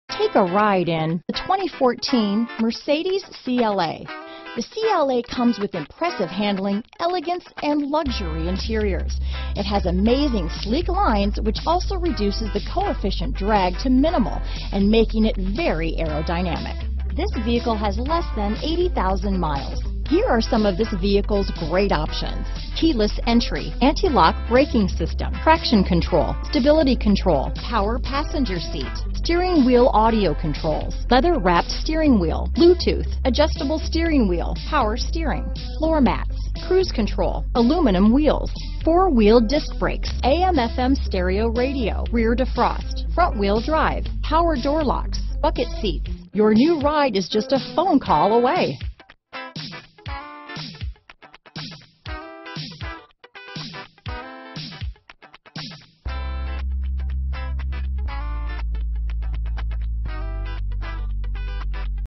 2014 Mercedes-Benz CLA-Class Atlanta, Griffin, McDonough, Fayetteville, Thomaston, GA U19377A
Used 2014 Mercedes-Benz CLA-Class available in Griffin Georgia at Cronic Chevrolet Buick GMC. Servicing the Atlanta, McDonough, Fayetteville, Thomaston, GA area.
2014 Mercedes-Benz CLA-Class CLA 250 - Stock#: U19377A - VIN#: WDDSJ4EB3EN044243

Clean CARFAX.Please call before coming down to look at a vehicle, due to our AMAZING PRICING they go quickly. 26/38 City/Highway MPGAwards:* ALG Residual Value Awards